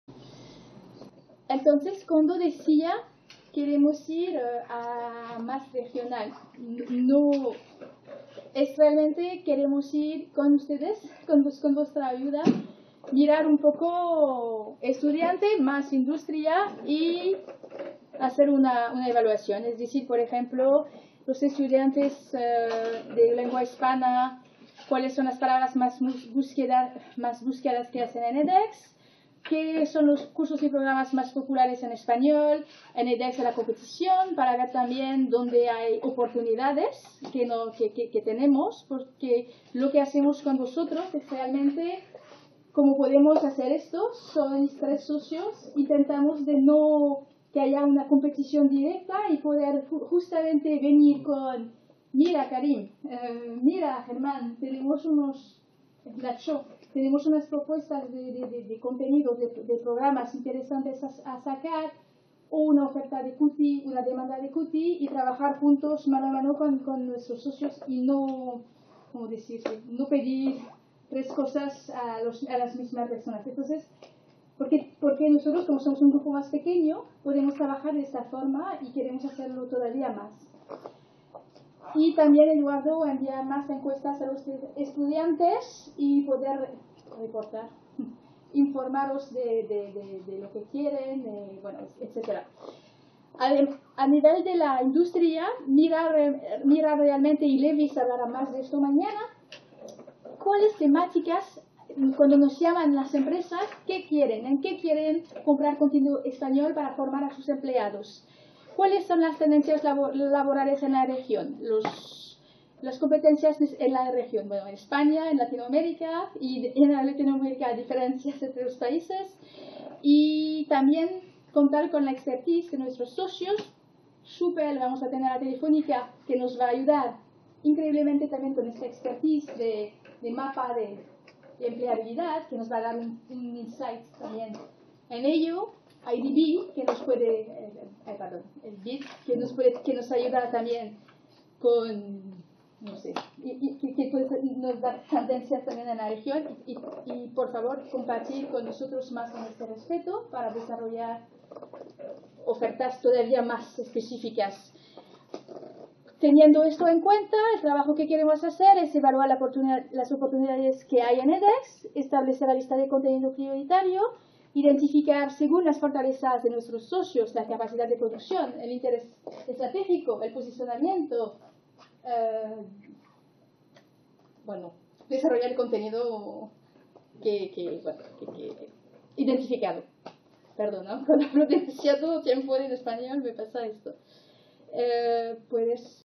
SPOC Gestión de MOOC. Estrategias de selección de temática para nuevos MOOC de edX| UPV
Título: SPOC Gestión de MOOC.  Estrategias de selección de temática para nuevos MOOC de edX

Descripción automática: En este video, se discute la estrategia para identificar oportunidades en la educación y la creación de contenido en español. Se menciona la importancia de entender las necesidades de los estudiantes, las palabras y búsquedas más frecuentes, y los programas educativos más populares. Se busca colaborar sin competencia negativa con socios potenciales para crear ofertas educativas demandadas y trabajar conjuntamente para ofrecer formación relevante.

Se habla también de responder a las necesidades de empresas interesadas en capacitar a sus empleados en español, adaptándose a las tendencias laborales y competencias requeridas en España y Latinoamérica. Para ello, se considera trabajar según las fortalezas de los socios y su capacidad de evolución, con el fin de desarrollar el contenido adecuado y maximizar el impacto estratégico y de posicionamiento.

Autor/a: Bret Marion